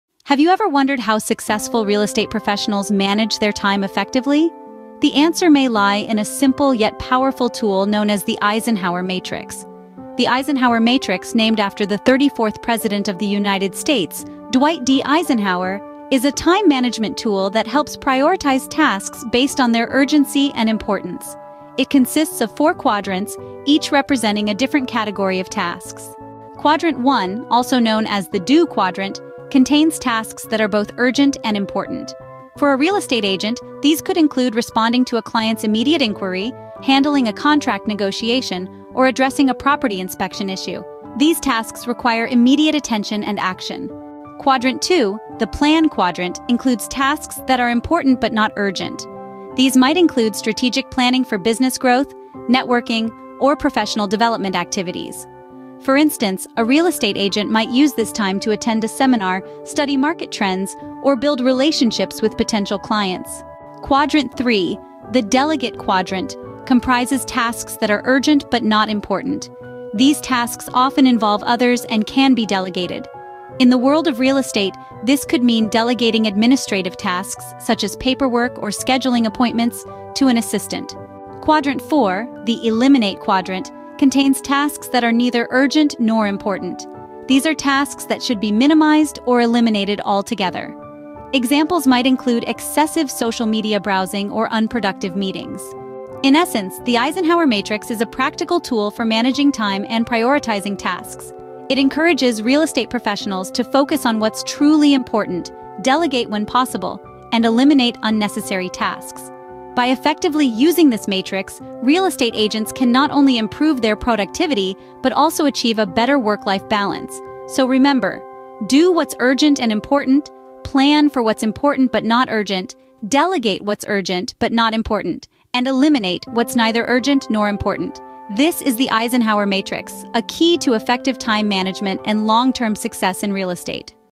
Unlock Productivity: Real Estate Agents' Guide to the Eisenhower Matrix
🏡 Are you a real estate professional looking to skyrocket your productivity and manage your time like a pro? Dive into our latest video, 'Mastering Time Management: The Eisenhower Matrix for Real Estate Agents.' This insightful guide is tailored specifically for the fast-paced world of real estate. Learn how to prioritize your tasks effectively using the Eisenhower Matrix, a proven method for enhancing efficiency and focusing on what truly matters in your career. 🌟

Whether you're juggling client meetings, property showings, or strategic planning, this video will equip you with practical tips and real estate-specific examples to help you balance urgent tasks with your long-term growth objectives. Say goodbye to overwhelm and hello to a more organized, goal-oriented approach to your workday. 🚀

Perfect for real estate agents, property managers, and anyone passionate about mastering time management in the real estate industry. Watch now and take the first step towards transforming your professional life! 🌐